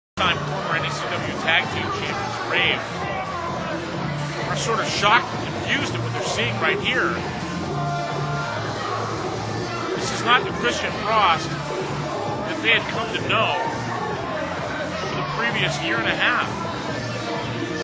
Fan Thrusting w/ Kristian Frost
At an NECW show, a fan decides to dance with "The Exotic" Kristian Frost. This isn't the kind of dance two guys should do together.